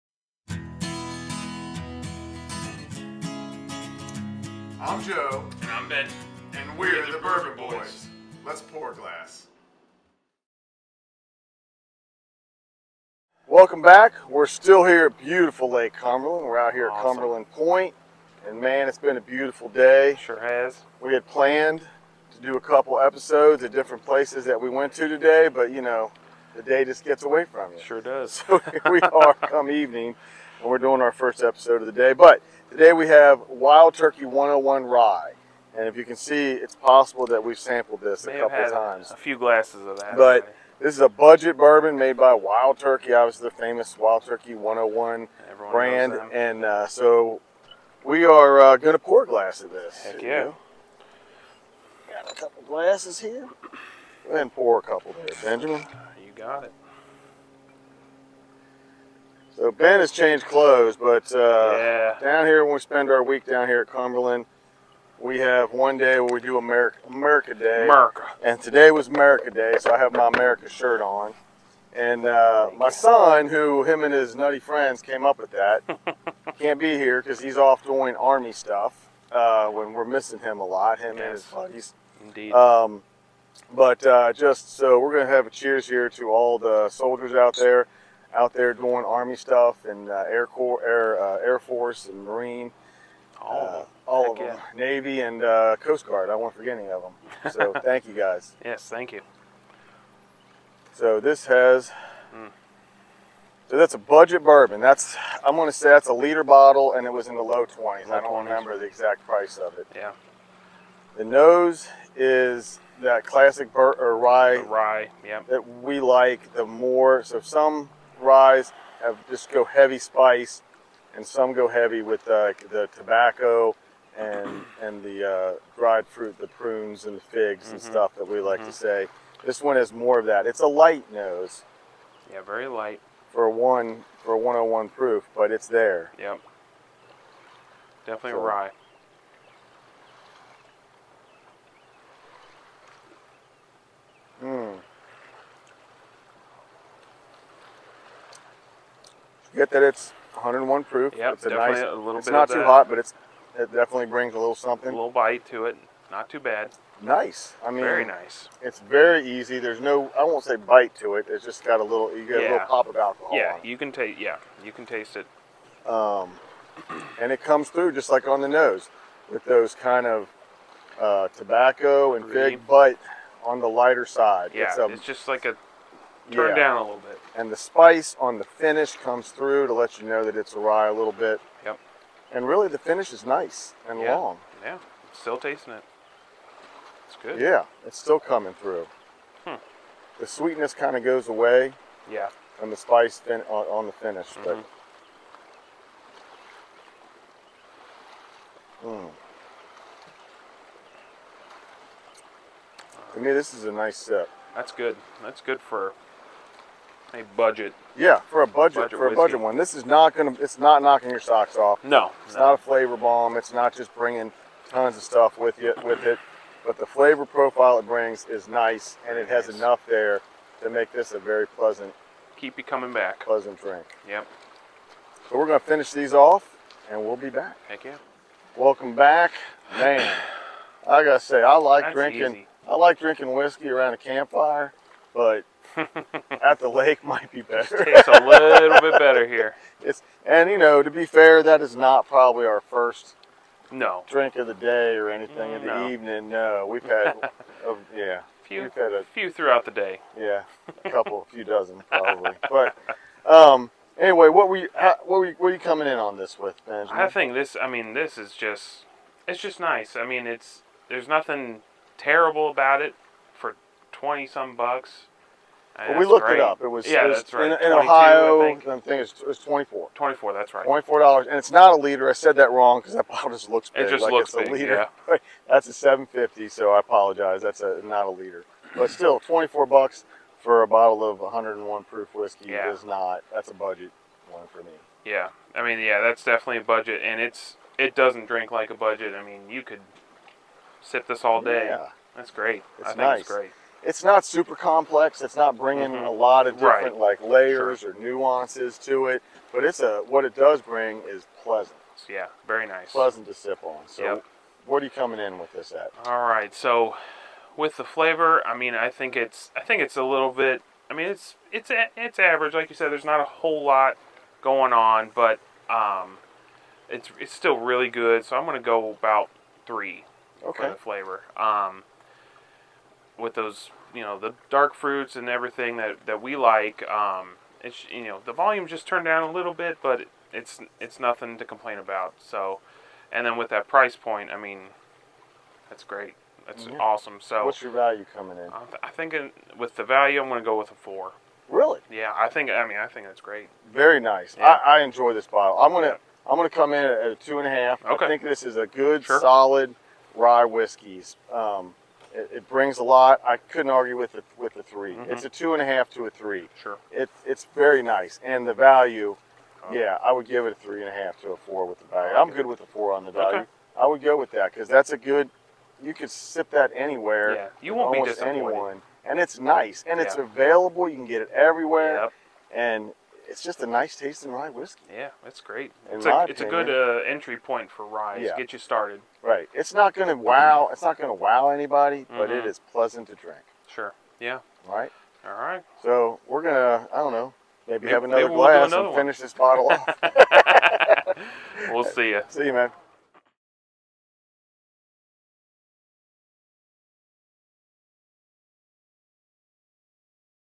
Bourbon Boys Review: Episode 65 - Wild Turkey 101 Rye
Another vide shot on beautiful Lake Cumberland. This week we are trying out another budget whiskey from Wild Turkey. 101 Rye comes in the mid 20s for price and delivers some great flavor for such a cheap whiskey. Check out the video and see what we think about it.

Till next week, happy sipping!